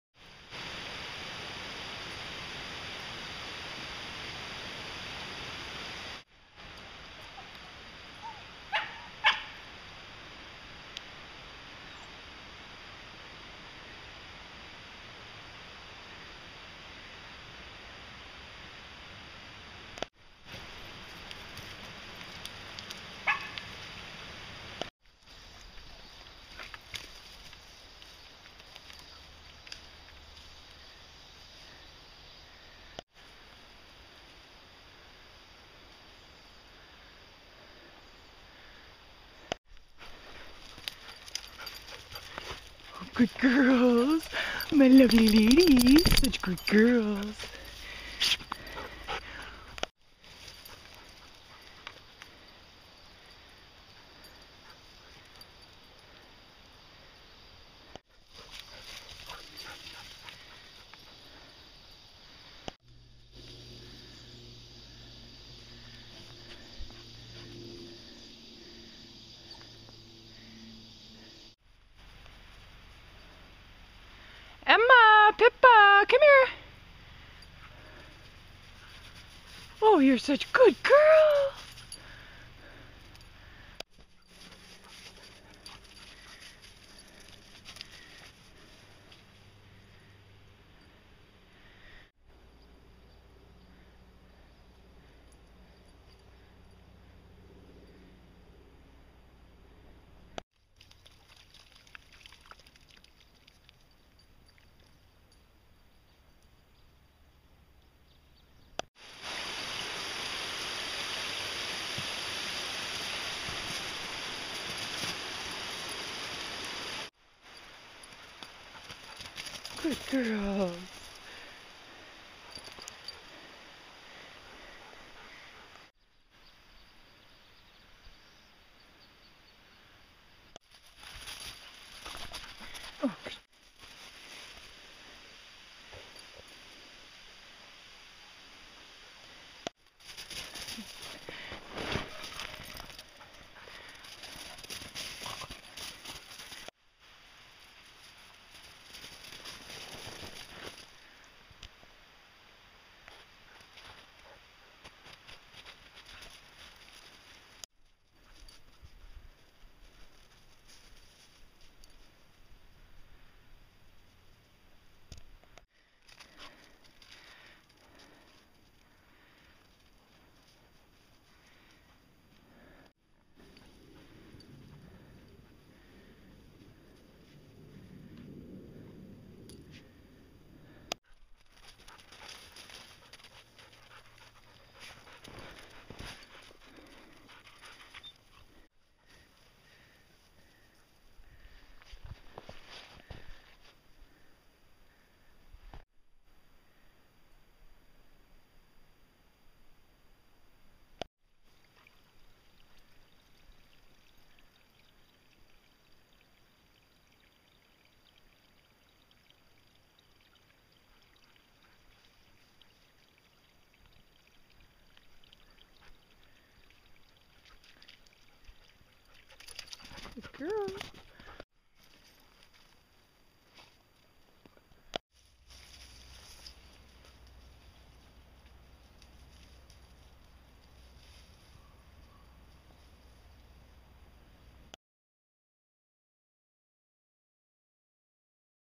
2012-03-17 Emma loves Pippa
Today was Emma and Pippa's second dog hike together. Wherever Pippa went, Emma was sure to follow. They really liked each other's company. It's so nice to see.